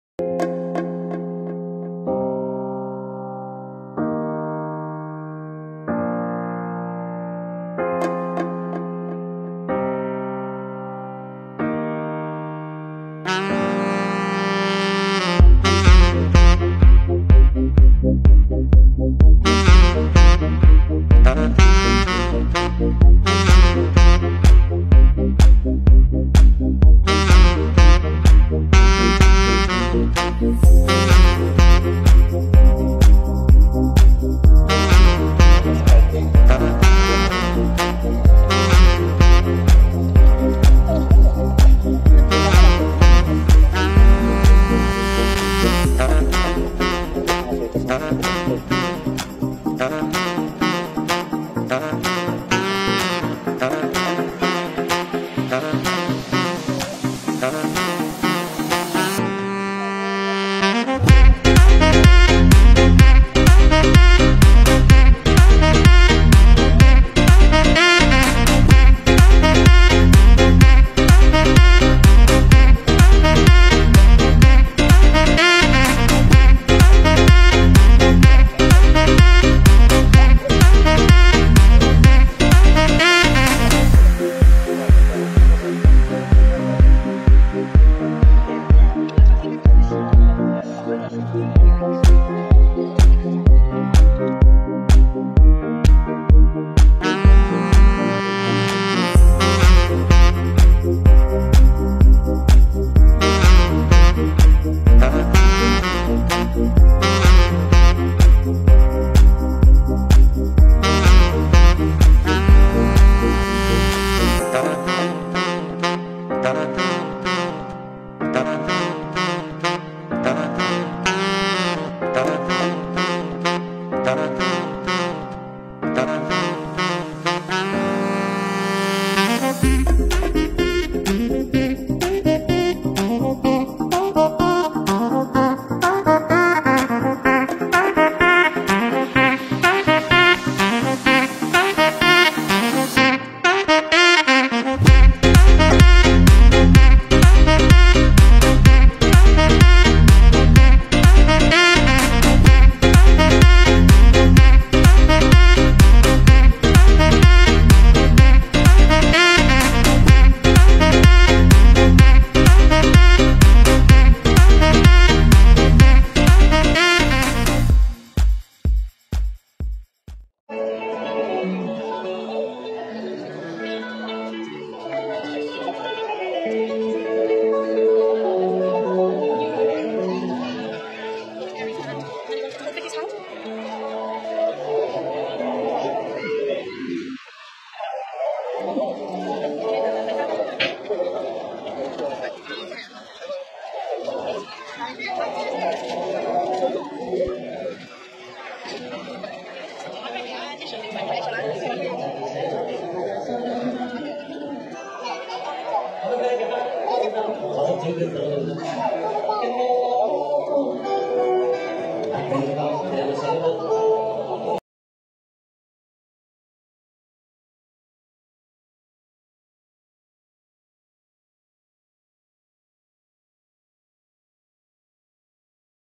4K walk in winchester -Gopro11-Have a coffee and enjoy this video and the beautiful music.
If you like this video,😋 comment, and